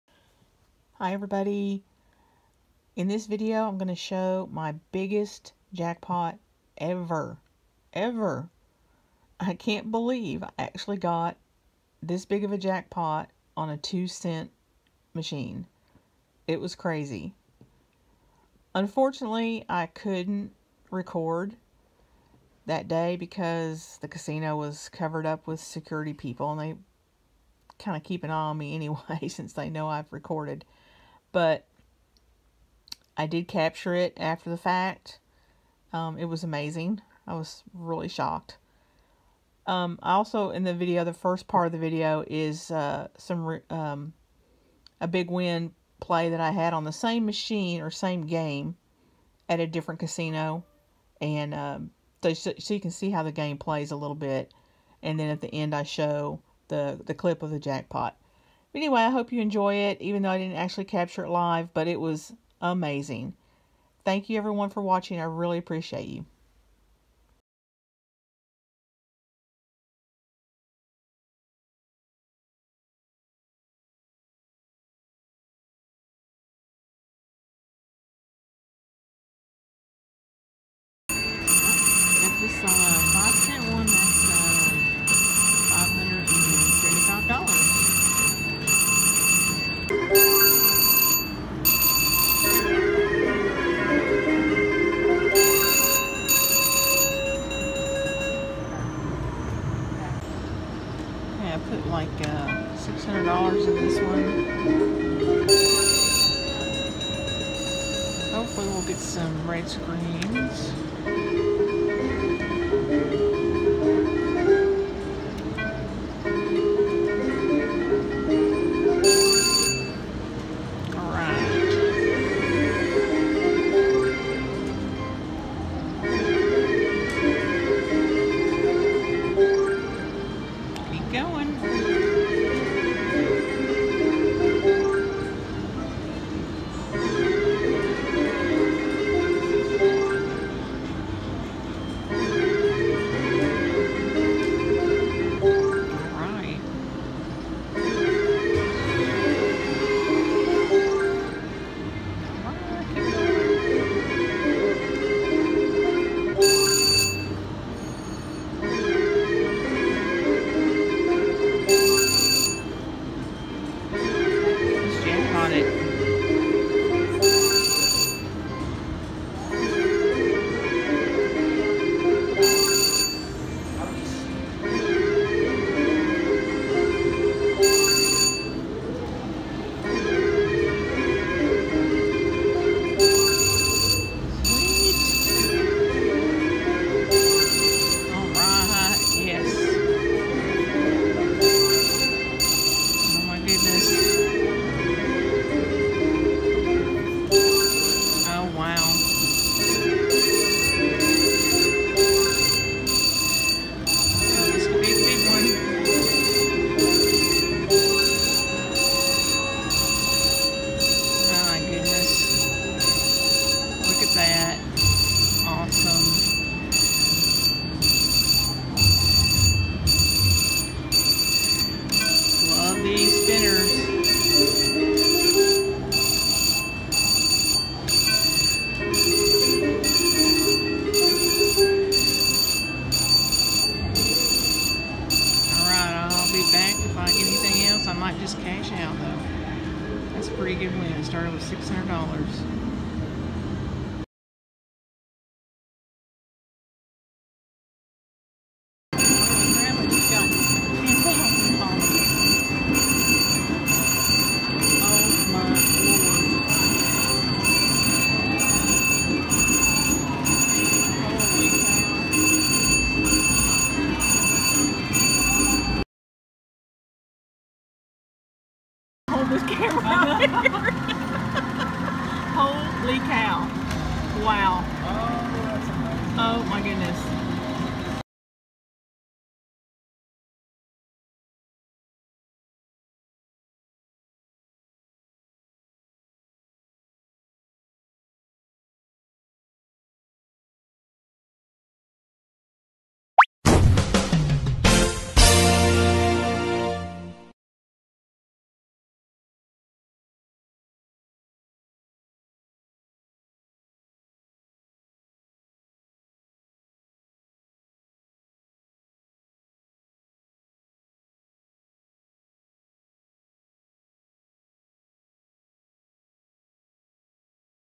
MY BIGGEST HANDYPAY EVER!! AMAZING, HUGE WIN!
I was playing a 2 cent multi-line VGT game called Golden Dolphin.
I had already made $100 profit off this machine and came back to give it another try. Boy, was that a lucky decision!! This is my biggest handpay EVER! I have won $10,000 before but this one is bigger than that! :)